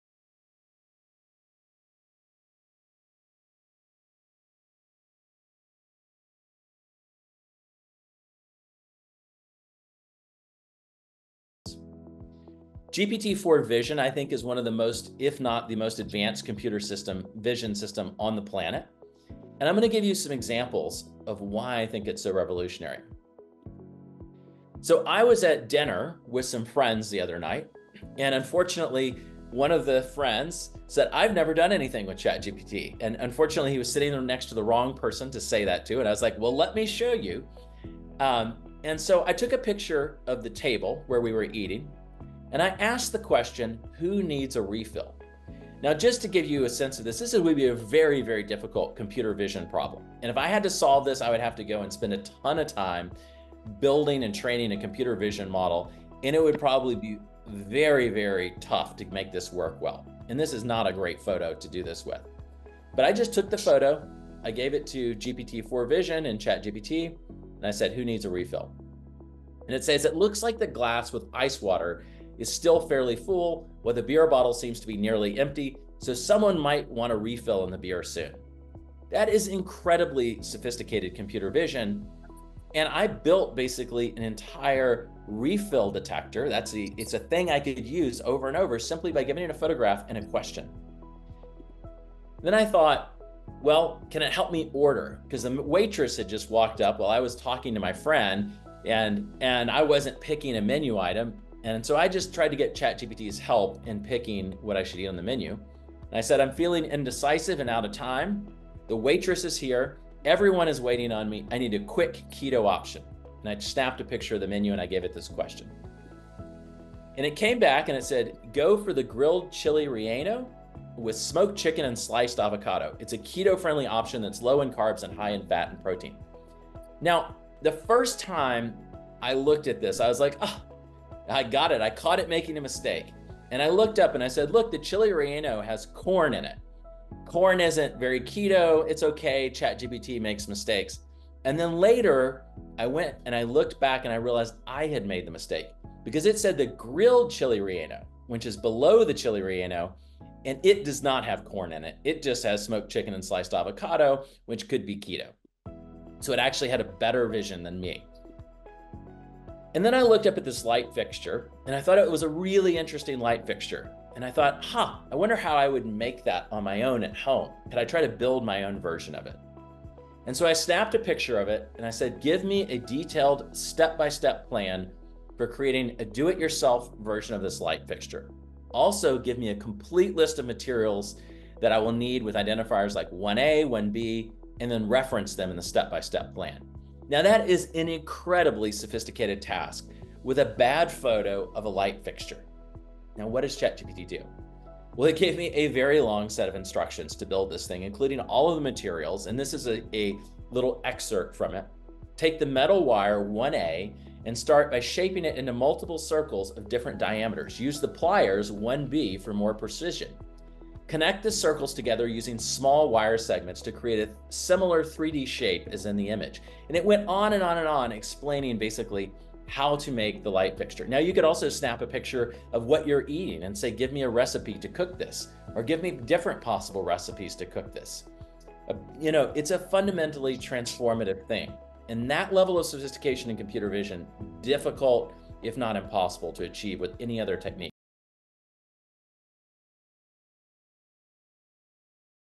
Generative AI Highlight #3: Image Analysis (Jules White)
Dr White, not just an acclaimed academic but also the creator of a massively popular Prompt Engineering course on Coursera, guides us through the vast potential of Generative AI – from personalised learning to cybersecurity.